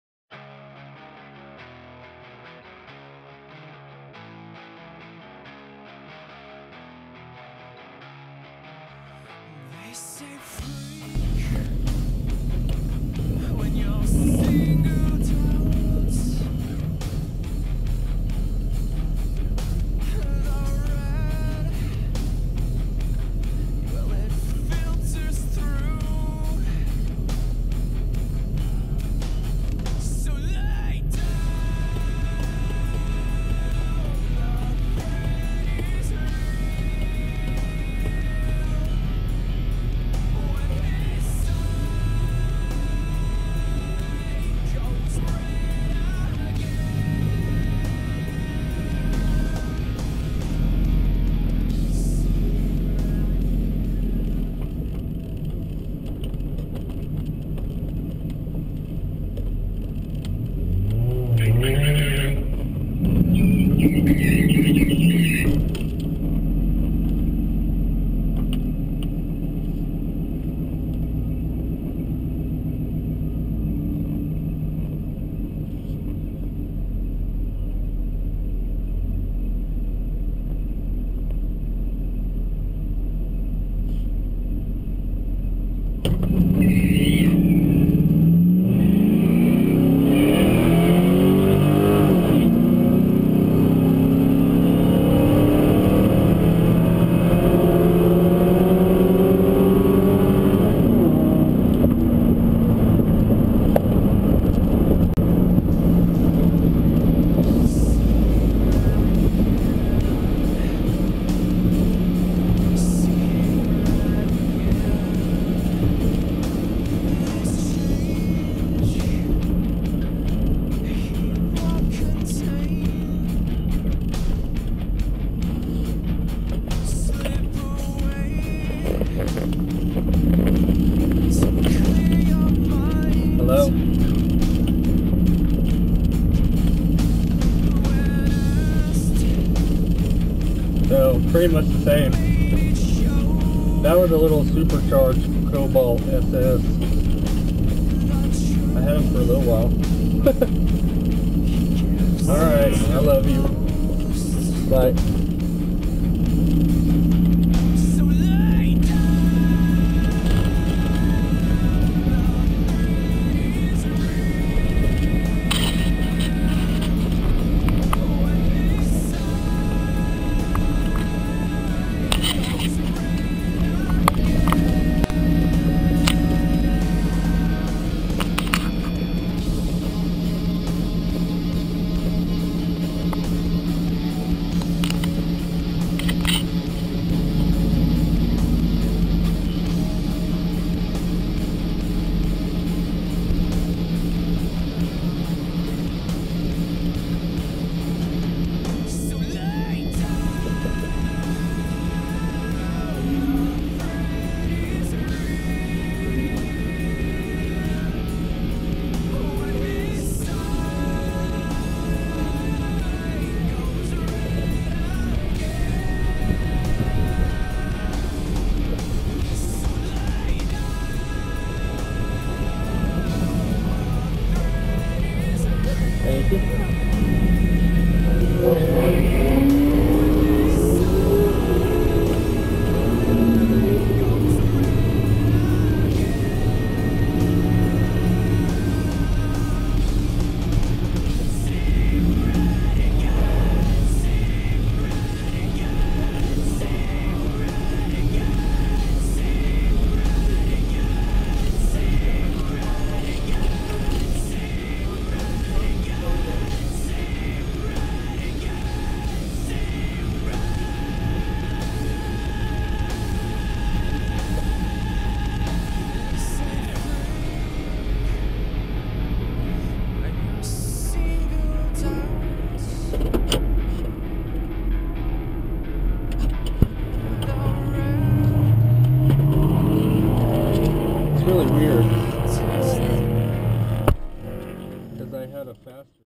My Movie
cammed 4.8 , baseline run before turbo  15.2 at 93mph.  3.42s , no stall 2.4460ft